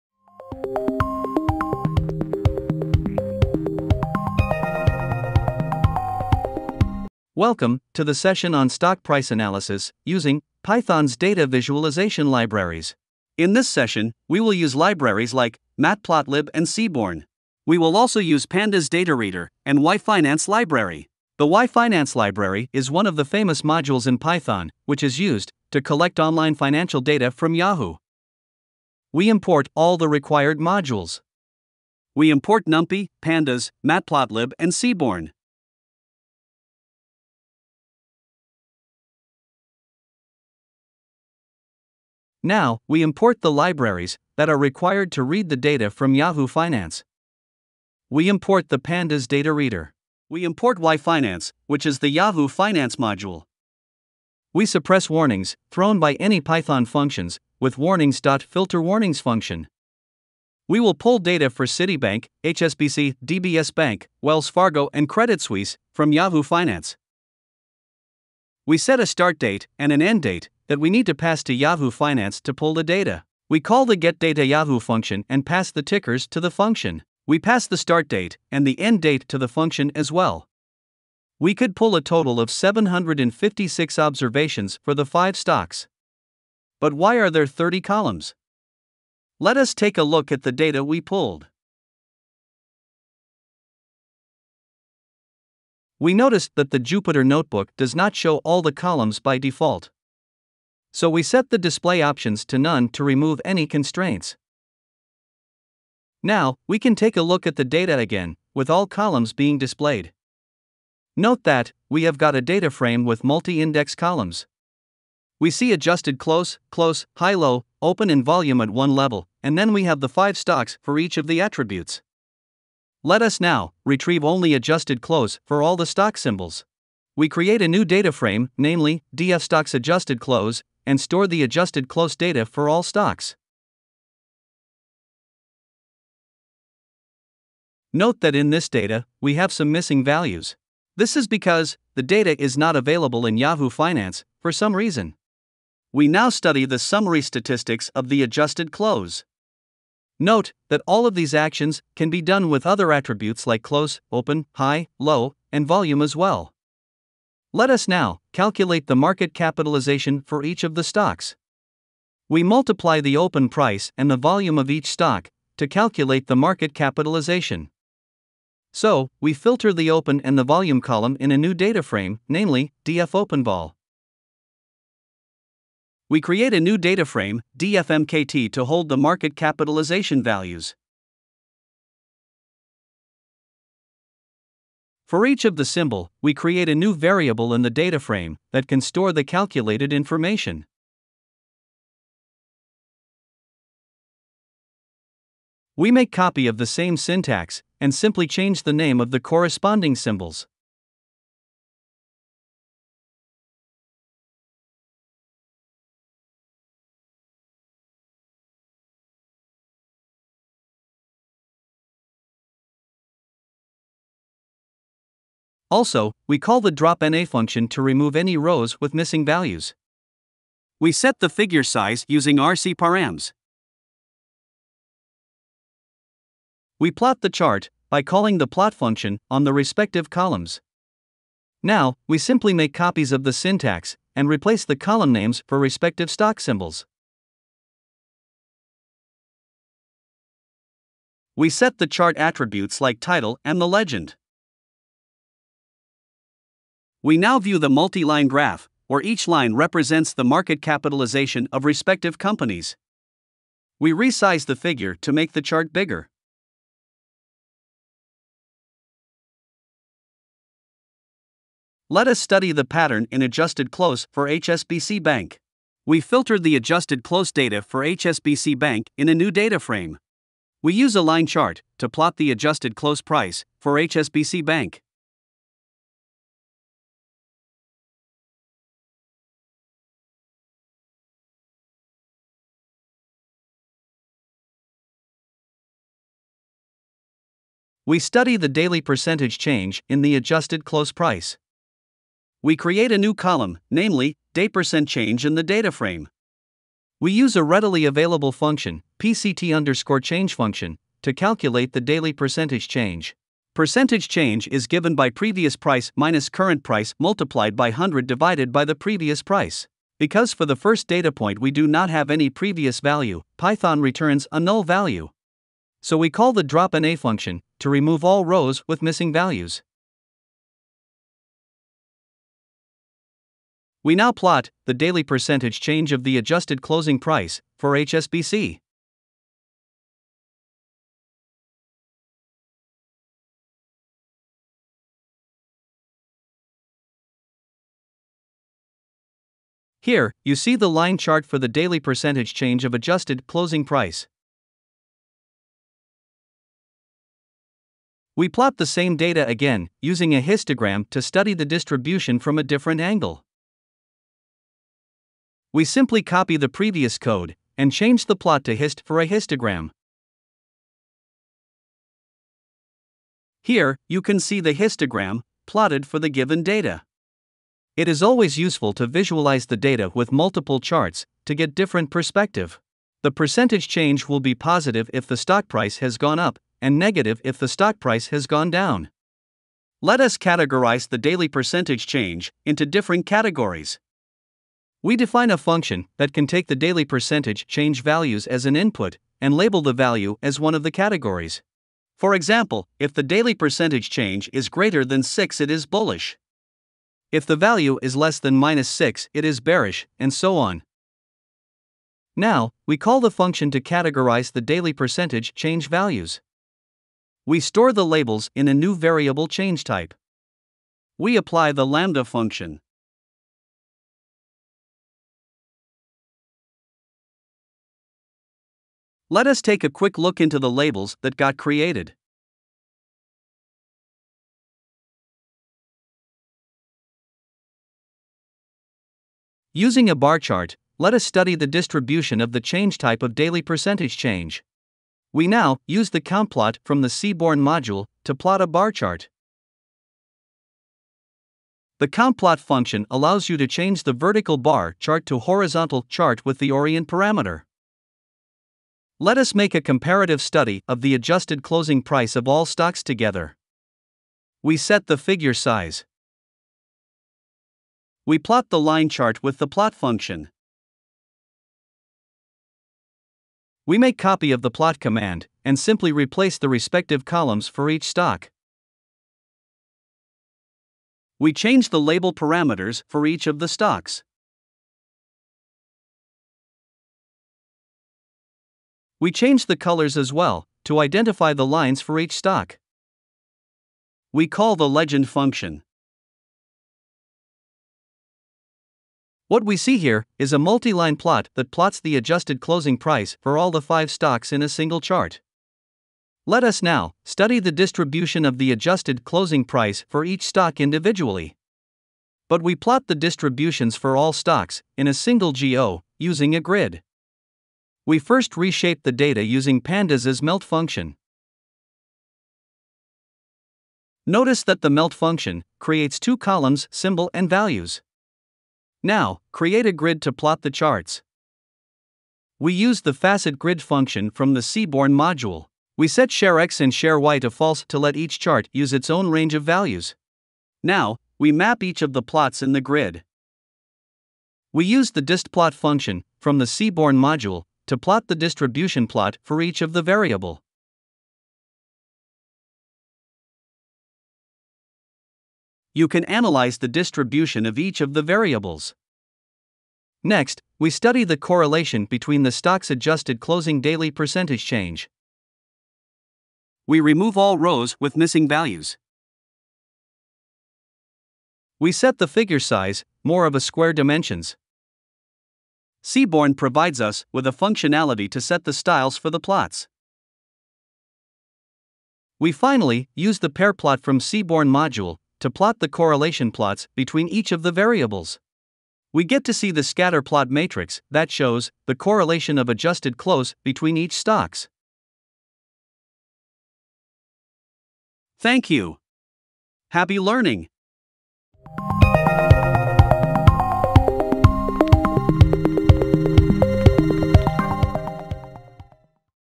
Stock Price Analysis | Python | Data Visualization | Yahoo Finance
This video is for intermediate learners who understand the basics of Python and have used Matplotlib & Seaborn earlier. This video focuses on how to manipulate a multi-index data frame and use the same for different types of plots for stock price analysis. Stock price analysis involves comparing a company's current financial statement to its financial statements in previous years to give an investor a sense of whether the company is growing, stable, or deteriorating.